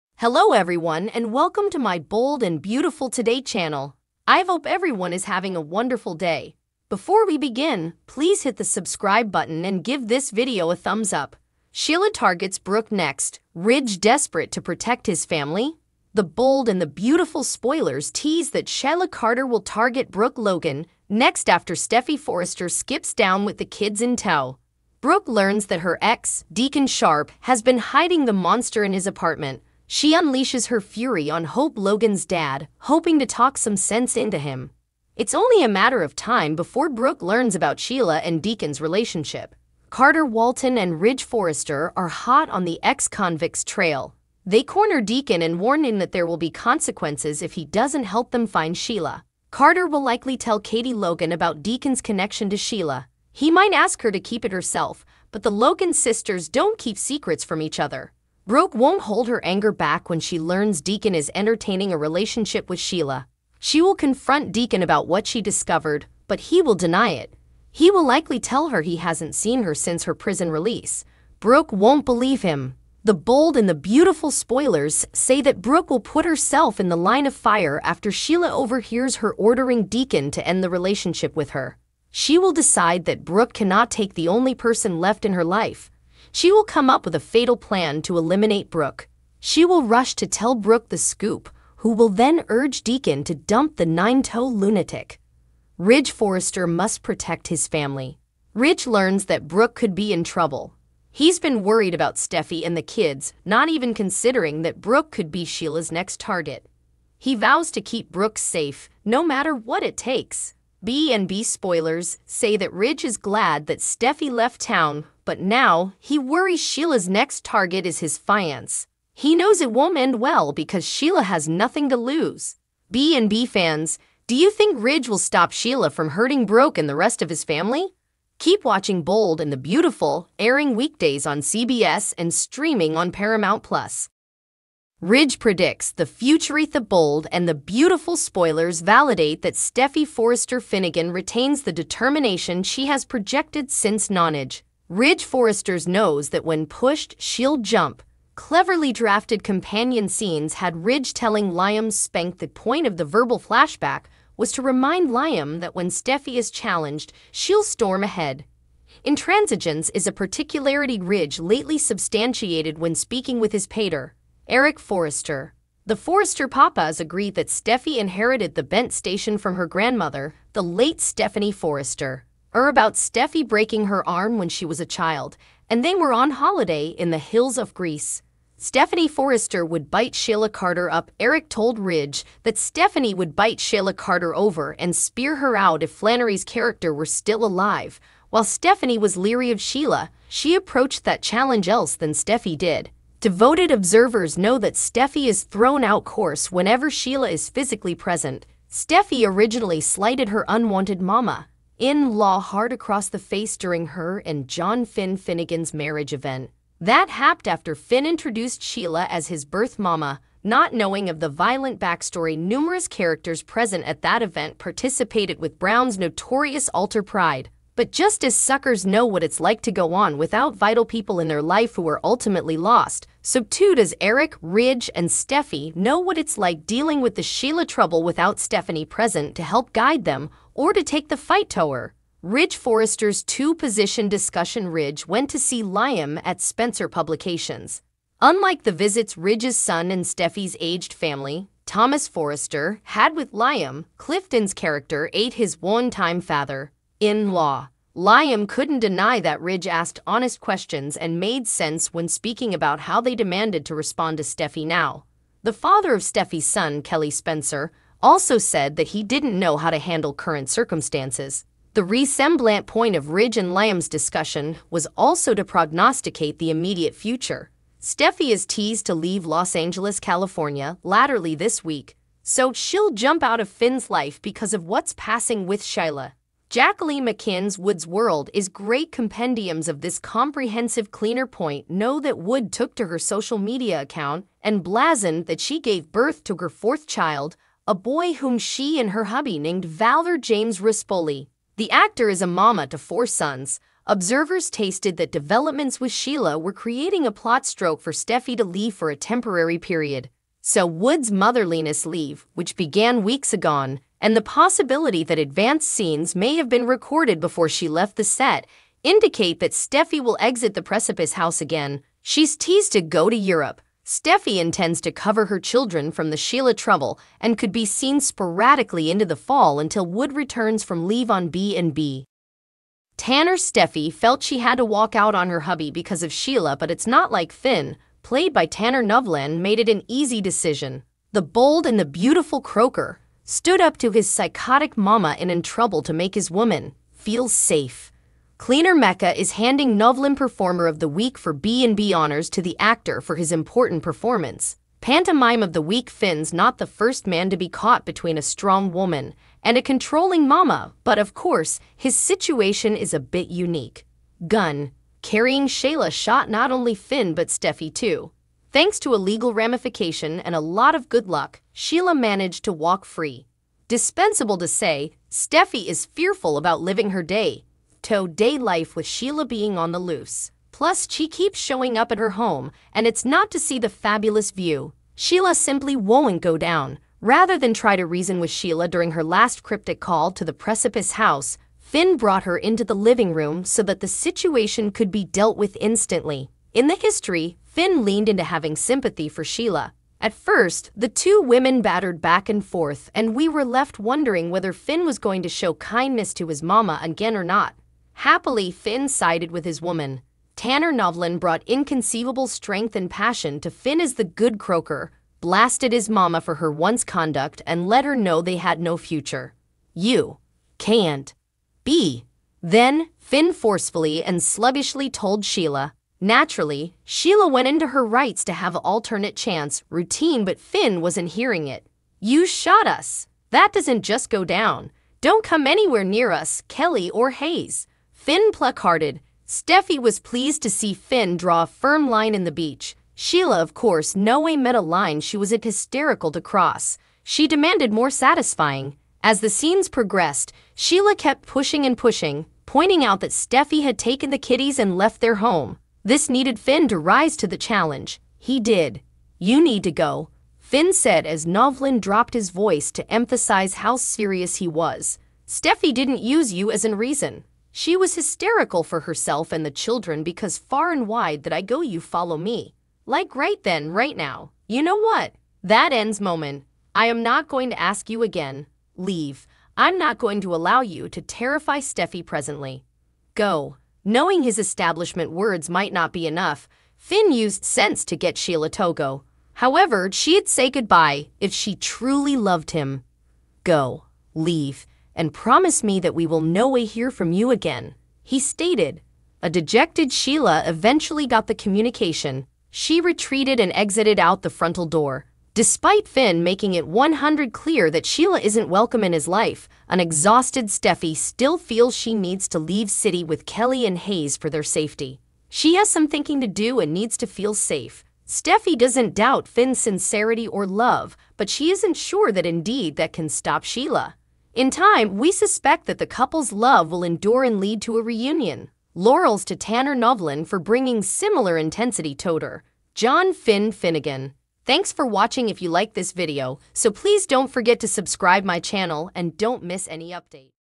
"Ridge's Desperate Battle: Protecting Brooke from Sheila's Vendetta"
"Sheila's New Target Revealed: Brooke in the Crosshairs!"
"Brooke Under Siege: Sheila's Shocking Scheme Unveiled"
"Sheila Strikes Again: Brooke Faces Terrifying Threat"
"Ridge's Worst Nightmare: Sheila's Vendetta Against the Forresters Continues"